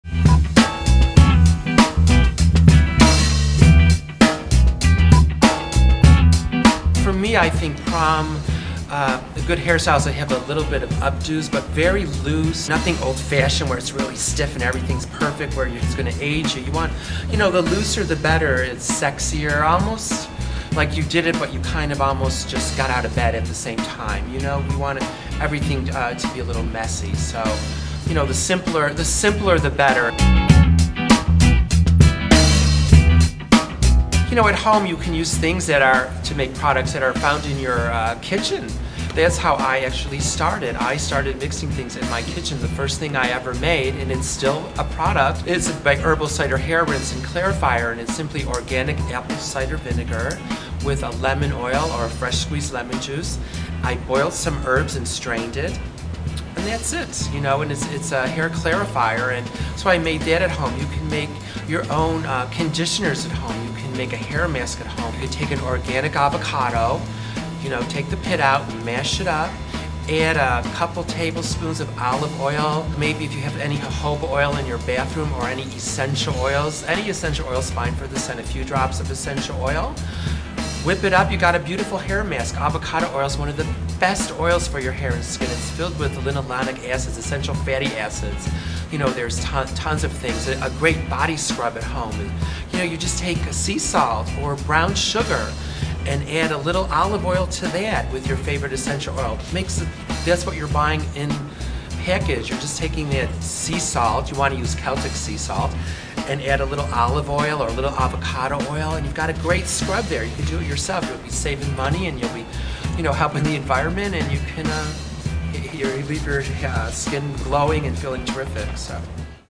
Project Green Prom Expert Vlog: John Masters
John Masters, owner and founder of John Masters Organics, a line of sustainable hair products, give tips on how to green your hairstyle, and what styles look good this prom season.